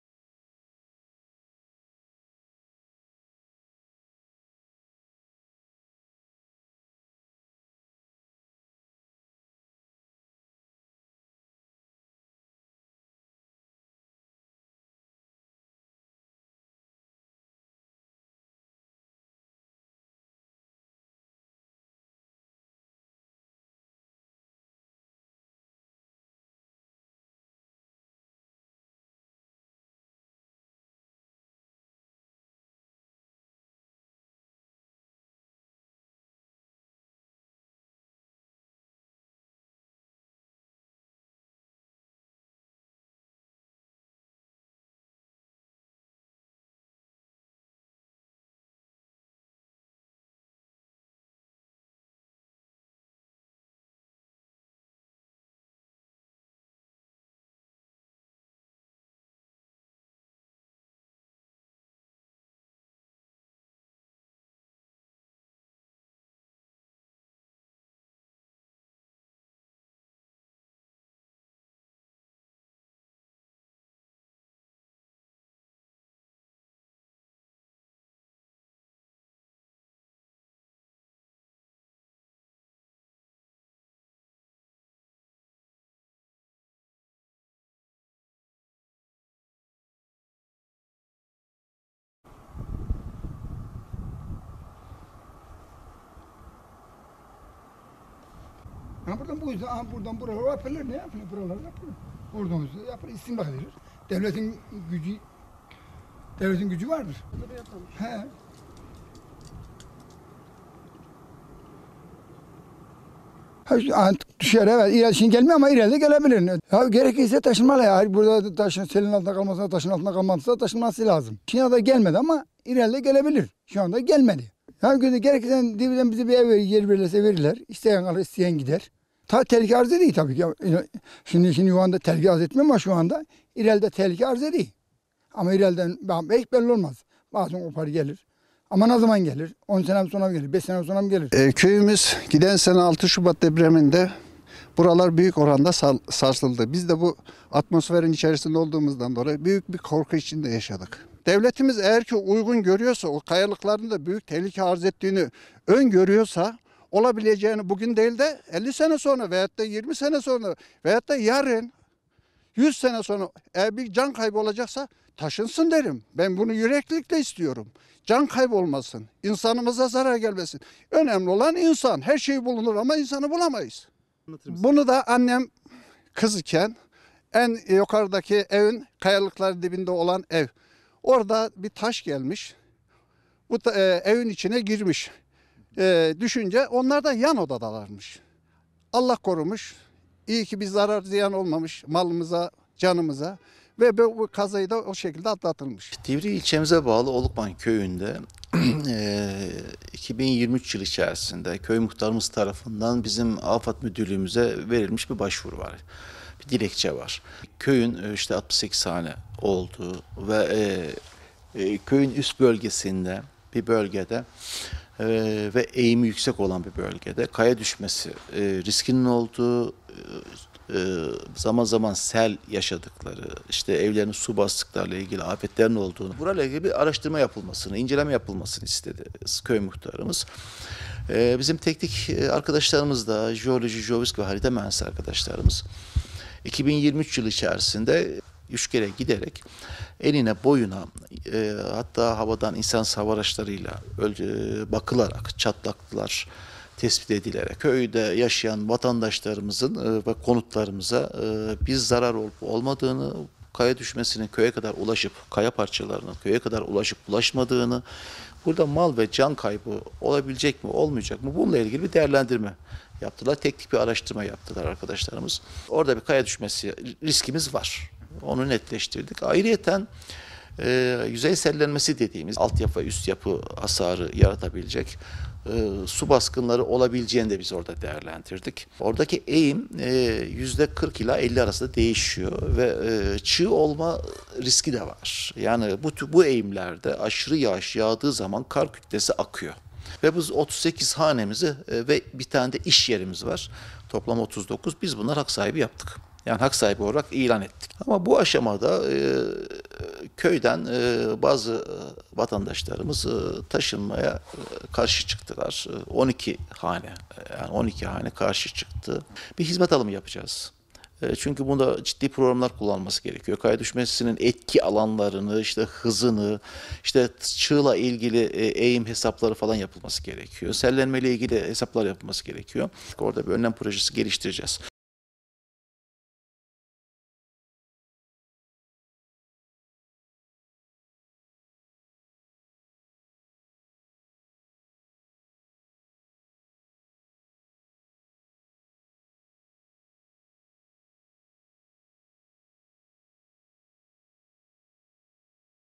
Dev mantar kayalar köyü tehdit ediyor
Sivas’ın Divriği ilçesinde bulunan Olukman köyü, köyün yamacında bulunan dev mantar kayaların tehdidi altında, köy sakinleri geve uyku uyuyamazken AFAD tarafından önleyici çalışma başlattı.
Sivas’ın Divriği ilçe merkezine 17 kilometre uzaklıkta bulunan Olukman köyü, yamacında yer alan dev mantar kayaların tehdidi altında. Köyün eteğinde kurulduğu dağın yüzeyinde bulunan dev mantar kayalar köy sakinleri için tehlike arz ediyor. 2023 yılında meydana gelen asrın felaketi köy sakinlerinin endişesini daha da arttırdı. Köy muhtarı Muhsin Erdoğdu tedbir alınması için İl Afet ve Acil Durum Müdürlüğü’nde (AFAD) dilekçe yazı. Köyün yanı başında bulunan kaya parçalarının köy sakinlerini tedirgin ettiğini ifade eden Erdoğdu, yetkililerden önlem almalarını istedi.
Konuyla ilgili harekete geçen AFAD İl Müdürlüğü ekipleri, köye giderek çeşitli incelemeler yaptı. Köy ve dağda yapılan ölçümler sonrası köyde bazı hanelerin taşınması gündeme geldi.
Olukman köyü muhtarı Muhsin Erdoğdu, deprem sonrası tedbir alınması için harekete geçtiklerini belirterek, “Geçen sene 6 Şubat depremlerinde buralar çok büyük oranda sallandı. Bu bölgede büyük bir korku içerisinde depremi yaşadık. Devletimiz bizim köyümüzde bulunan kayaların tehlike arz ettiğini öngörüyorsa, bir can kaybı yaşanabilecekse taşınsın derim. Ben bunu yüreklilikle isterim. Can kaybı olmasın, insanımıza zarar gelmesin, önemli olan insan. Annem gençken köyün en yukarısında bulunan evde oturuyormuş. Evin içerisine bir taş düşmüş. Taş düştüğü anda annemler de yan odada bulunuyormuş. Allah korumuş, iyi ki bir can kaybı olmamış. Bu kaza da o şekilde atlatılmış” dedi.
68 haneli köyü incelediklerini ifade eden AFAD İl Müdürü Cezmi Türkmen, “Divriği ilçemize bağlı Olukman köyünde 2023 yılı içerisinde köy muhtarımız tarafından AFAD Müdürlüğü’müze verilmiş bir dilekçe var. Köyün 68 hane olduğu ve köyün üst bölgesinde eğimi yüksek olan bir bölgede kaya düşme riskinin yüksek olduğu, zaman zaman sel yaşadıklarını ifade eden dilekçe verildi. Köy muhtarımız buralar ile ilgili araştırma yapılmasını istedi. Bizim Jeoloji, Jeofizik ve Harita Mühendisi arkadaşlarımız 2023 yılı içerisinde 3 kere giderek enine boyuna İnsansız Hava Araçları ile bakılarak çatlaklar tespit edildi. Düşebilecek kayaların köye kadar ulaşıp ulaşmadığını herhangi bir durumda can kaybı olup olmayacağını araştırdı arkadaşlar. Orada bir kaya düşmesi riski olduğunu netleştirdi arkadaşlar. Altyapı ve üstyapı hasarına neden olabilecek su baskınları ile ilgili de değerlendirmelerde bulunduk. Oradaki eğim, yüzde 40 ila 50 arasında değişiyor. Orada çığ olma riski de var. Bu denli eğimlerde aşırı yağış olduğunda kar kütlesi kayıyor” diye konuştu.
“Vatandaşlarımızı yerinden, yurdundan etmeyeceğiz”
Yapılan incelemeler sonrası 38 hane ve 1 işyerini hak sahibi yaptıklarını ifade eden İl Müdürü Türkmen, “Bu aşamada köyden 12 hane taşınmaya karşı çıktılar. Bu köy ile ilgili bir hizmet alımı yapacağız. Burası için ciddi programlar kullanılması gerekiyor. Kaya düşmesinin etki alanları, hızı, eğim hesapları vs. yapılması gerekiyor. Orada bir önlem projesi geliştireceğiz, vatandaşlarımızı yerinden yurdundan etmeyeceğiz. Divriği ilçemiz Olukman köyündeki sorunu kökünden çözeceğiz” ifadelerine yer verdi.